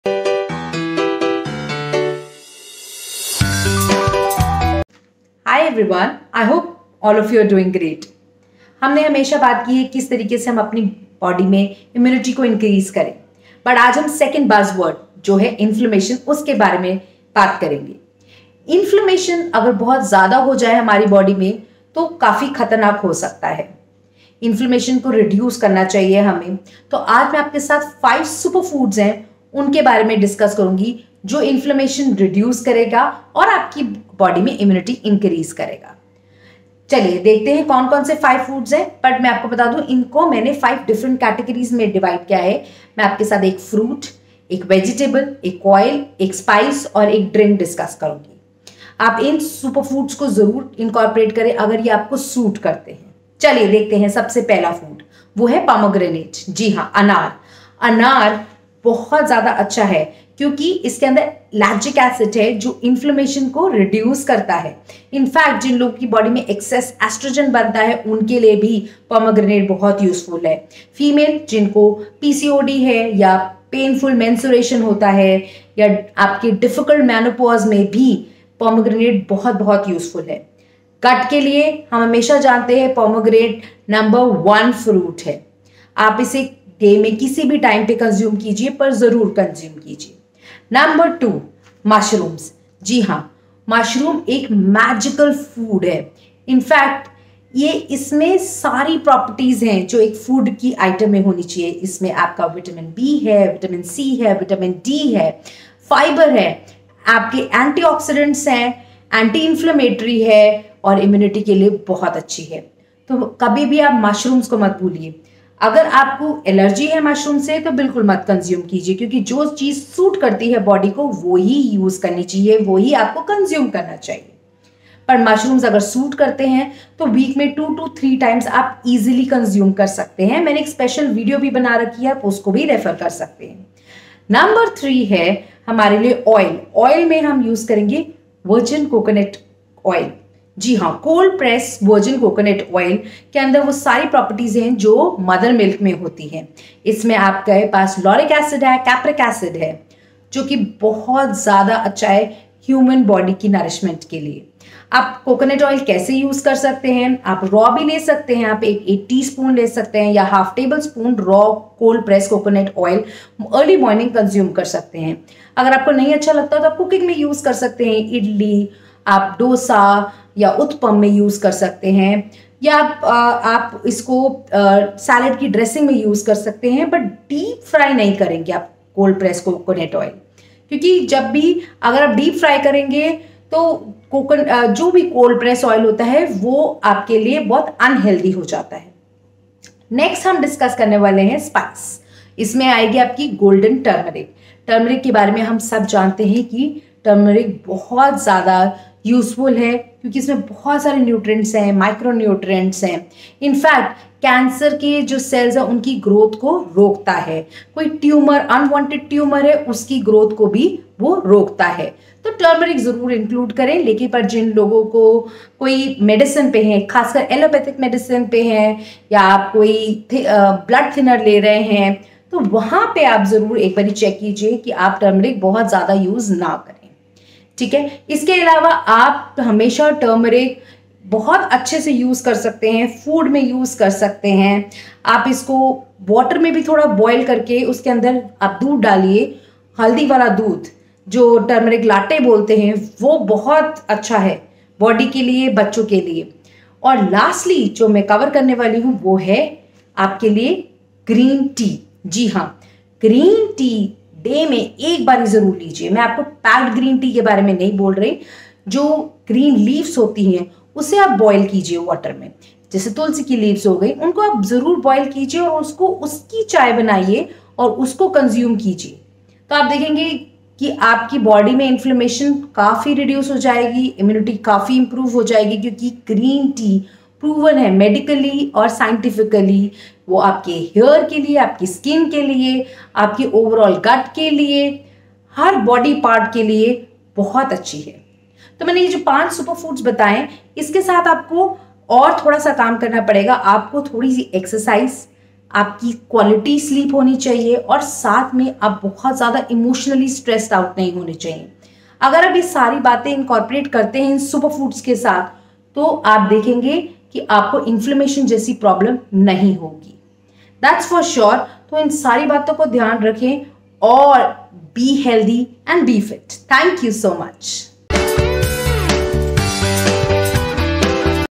5 Most Powerful Foods to Curb Inflammation
Enjoy below mentioned foods to prevent malnourishment in urban population:
1. Pomegranates
2. Mushrooms
3.Cold pressed Virgin Coconut oil
4. Turmeric
5. Green Tea